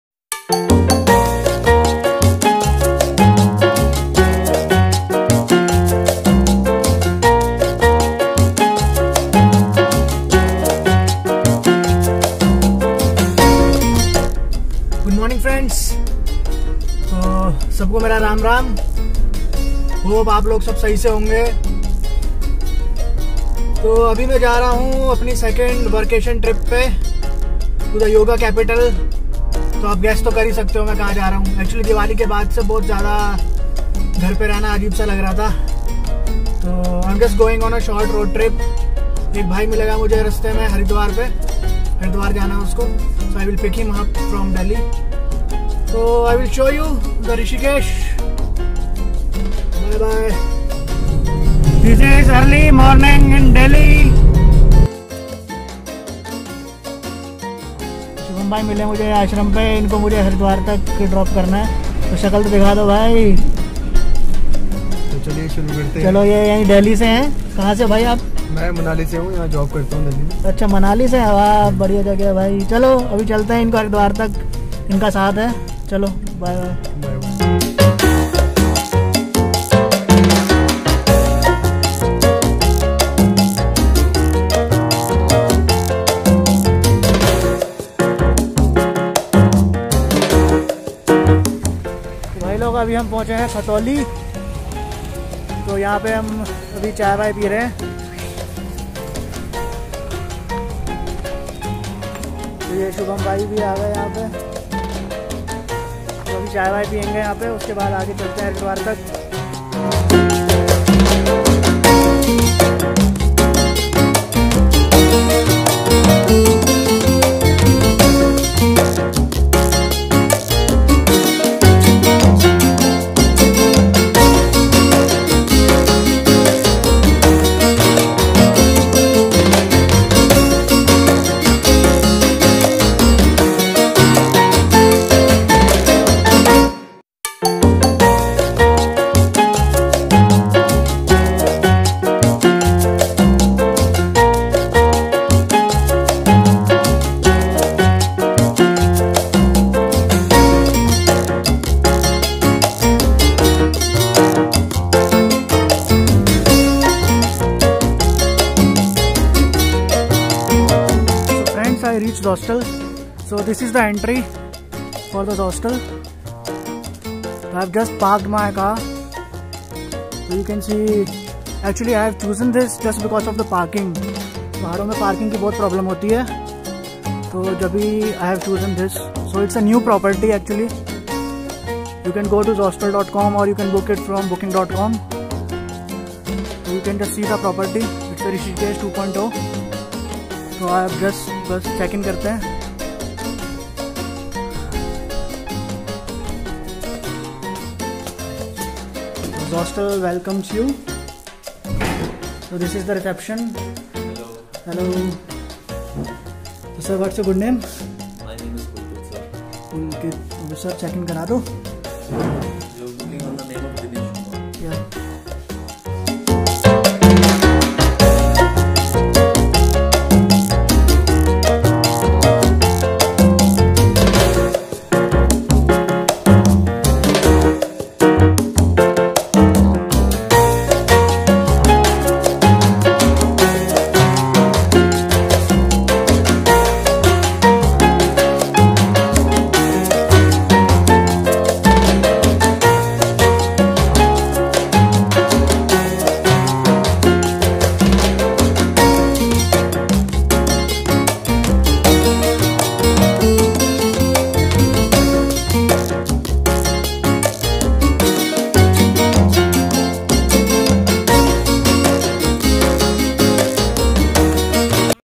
Rishikesh-Mussourie Vlog -Series (ep:01) 2nd Trip post Lockdown Fbd to Rishikesh short Road trip
hi all

this is my 2nd trip after lockdown to yoga capital,  this blog is only a road travel giving info about how to reach, time taken, where I stayed,

Keep following to see the famous Laxman khula, delicious food, Ganga Aarati etc